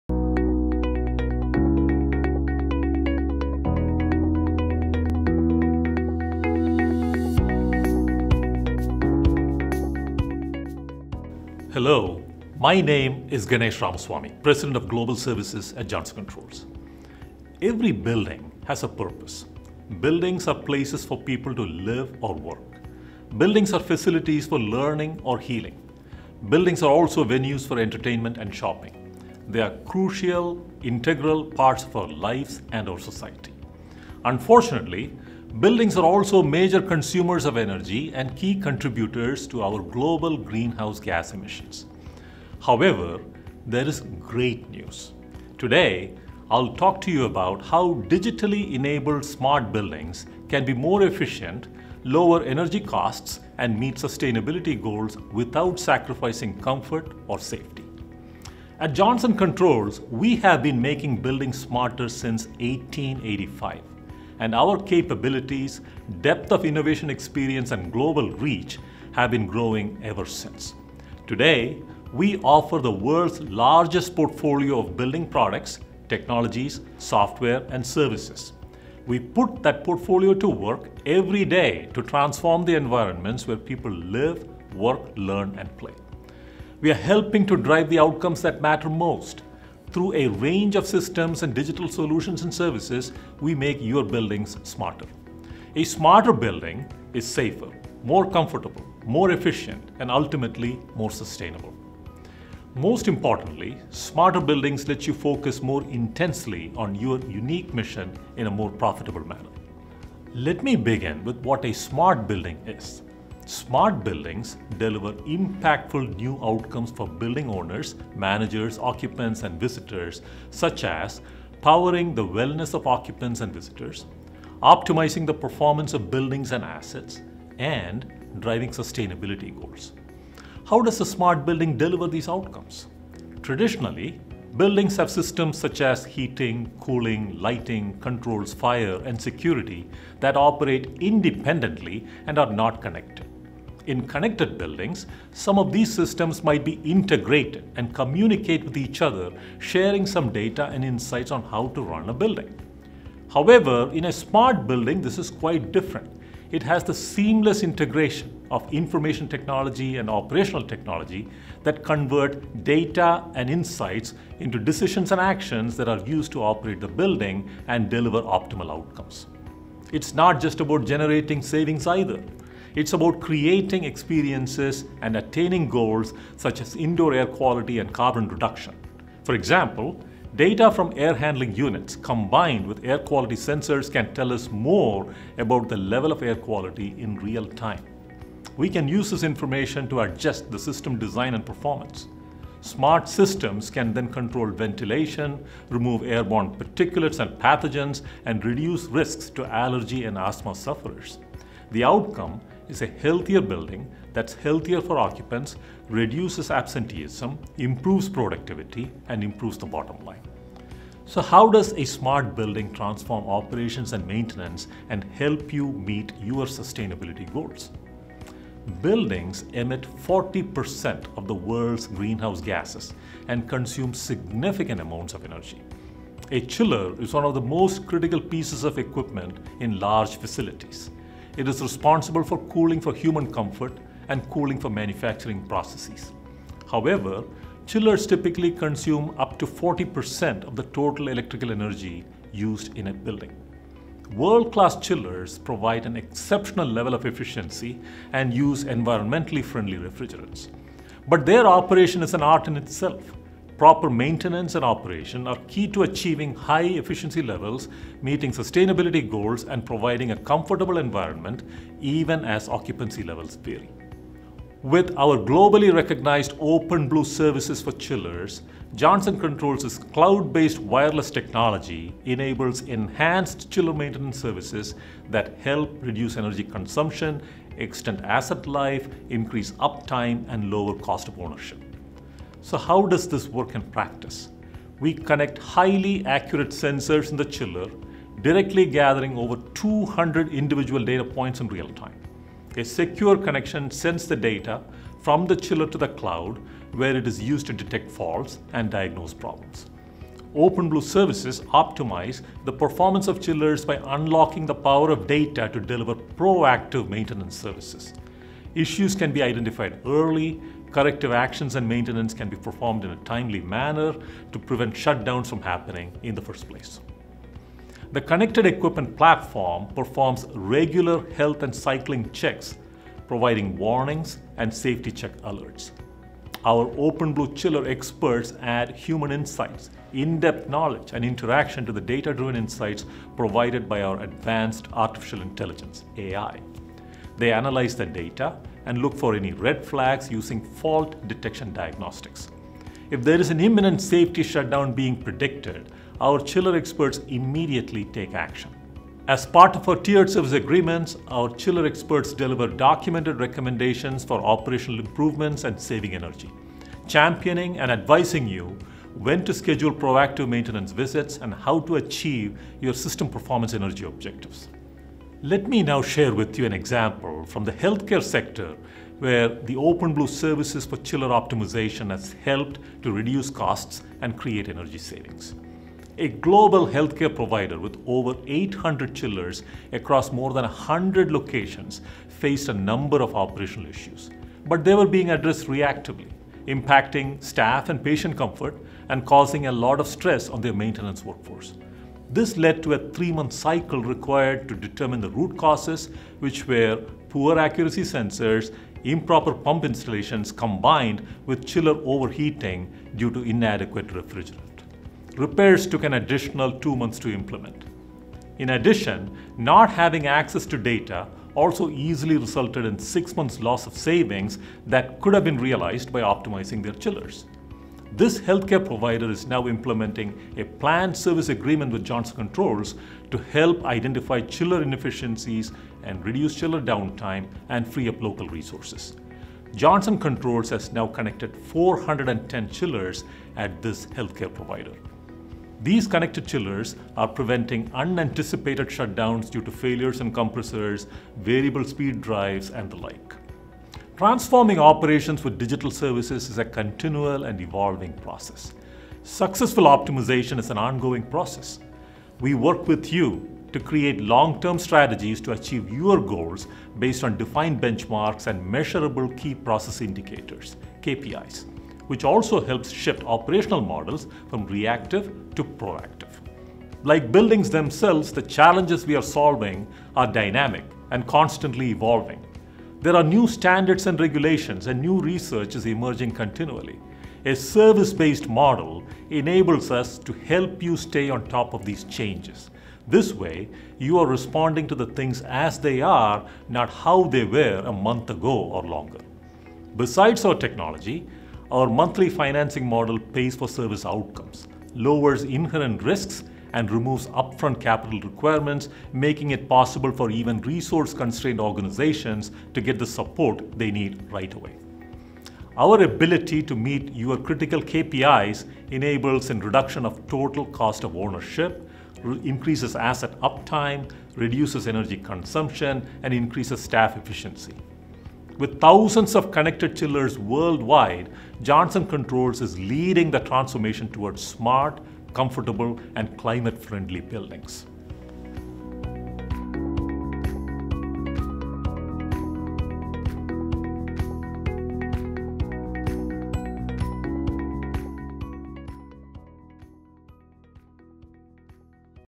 OpenBlue Services - Smart Building Technology and Optimizing Your Chiller Performance
Smart building technology and chiller optimization: Transform your operations and meet your financial and sustainability goals.

Buildings are critical, integral parts of our lives and our society. Unfortunately, buildings are also major consumers of energy and key contributors to global greenhouse gas emissions.

In this video, Ganesh Rameswamy, Vice President and President of Global Services at Johnson Controls, explains how digitally enabled smart buildings can be more efficient, lower energy costs and meet sustainability goals without sacrificing comfort or safety.